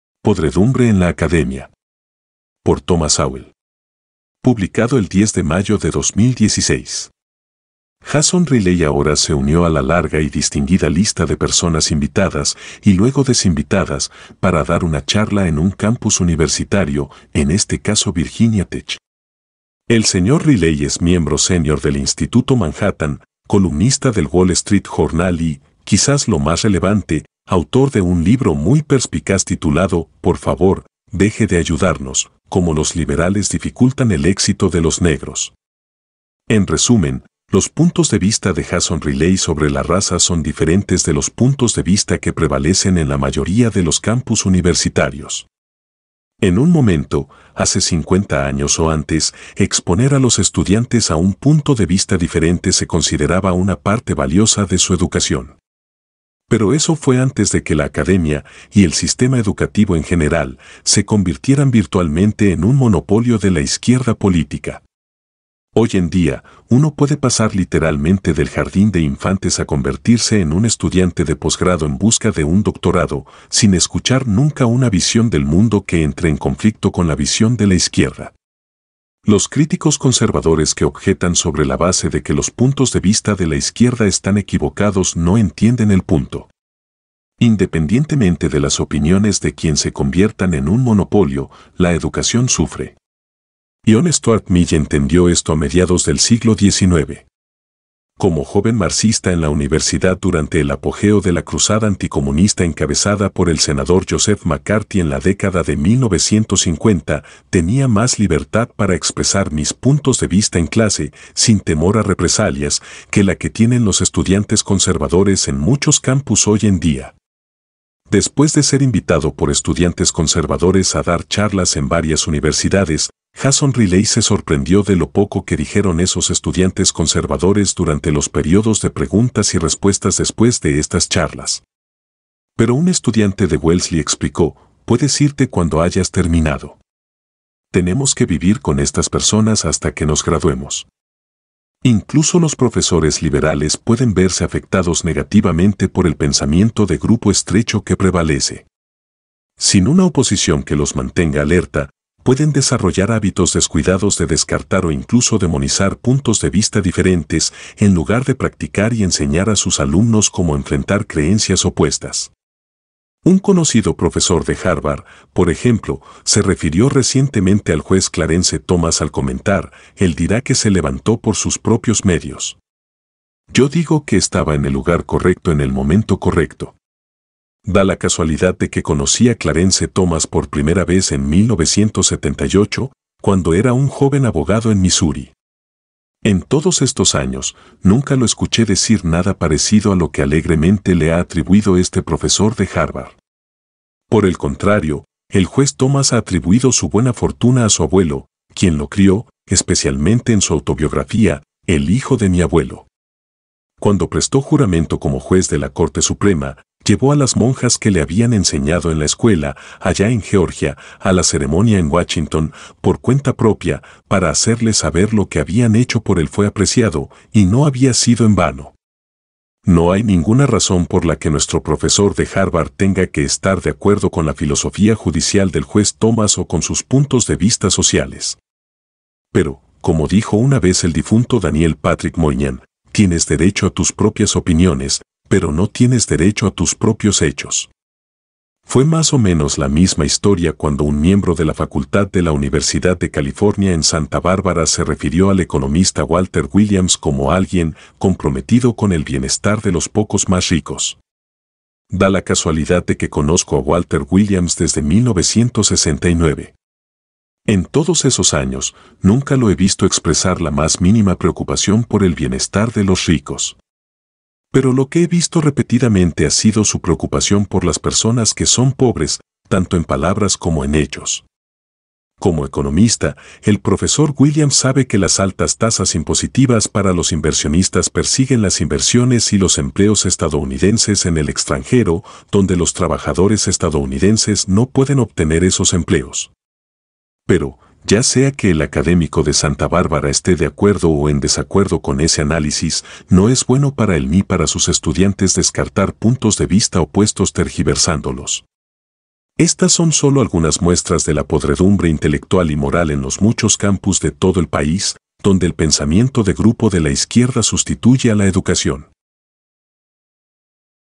Thomas Sowell - Podredumbre en la academia
PODREDUMBRE EN LA ACADEMIA

Por Thomas Sowell
Publicado el 10 de mayo de 2016

Jason Riley ahora se unió a la larga y distinguida lista de personas invitadas, y luego desinvitadas, para dar una charla en un campus universitario, en este caso Virginia Tech.

El Sr. Riley es miembro sénior del Instituto Manhattan, columnista del Wall Street Journal y, quizás lo más relevante, autor de un libro muy perspicaz titulado "Por favor, deje de ayudarnos: cómo los liberales dificultan el éxito de los negros".

En resumen, los puntos de vista de Jason Riley sobre la raza son diferentes de los puntos de vista que prevalecen en la mayoría de los campus universitarios. En un momento, hace 50 años o antes, exponer a los estudiantes a un punto de vista diferente se consideraba una parte valiosa de su educación. Pero eso fue antes de que la academia, y el sistema educativo en general, se convirtieran virtualmente en un monopolio de la izquierda política.

Hoy en día, uno puede pasar literalmente del jardín de infantes a convertirse en un estudiante de posgrado en busca de un doctorado, sin escuchar nunca una visión del mundo que entre en conflicto con la visión de la izquierda.

Los críticos conservadores que objetan sobre la base de que los puntos de vista de la izquierda están equivocados no entienden el punto. Independientemente de las opiniones de quién se conviertan en un monopolio, la educación sufre. John Stuart Mill entendió esto a mediados del siglo XIX.

Como joven marxista en la universidad durante el apogeo de la cruzada anticomunista encabezada por el senador Joseph McCarthy en la década de 1950, tenía más libertad para expresar mis puntos de vista en clase, sin temor a represalias, que la que tienen los estudiantes conservadores en muchos campus hoy en día.

Después de ser invitado por estudiantes conservadores a dar charlas en varias universidades, Jason Riley se sorprendió de lo poco que dijeron esos estudiantes conservadores durante los períodos de preguntas y respuestas después de estas charlas. Pero un estudiante de Wellesley explicó: "Puedes irte cuando hayas terminado. Tenemos que vivir con estas personas hasta que nos graduemos".

Incluso los profesores liberales pueden verse afectados negativamente por el pensamiento de grupo estrecho que prevalece. Sin una oposición que los mantenga alerta, pueden desarrollar hábitos descuidados de descartar o incluso demonizar puntos de vista diferentes, en lugar de practicar y enseñar a sus alumnos cómo enfrentar creencias opuestas.

Un conocido profesor de Harvard, por ejemplo, se refirió recientemente al juez Clarence Thomas al comentar: "Él dirá que se levantó por sus propios medios. Yo digo que estaba en el lugar correcto en el momento correcto".

Da la casualidad de que conocí a Clarence Thomas por primera vez en 1978, cuando era un joven abogado en Missouri. En todos estos años, nunca lo escuché decir nada parecido a lo que alegremente le ha atribuido este profesor de Harvard.

Por el contrario, el juez Thomas ha atribuido su buena fortuna a su abuelo, quien lo crió, especialmente en su autobiografía, "El hijo de mi abuelo".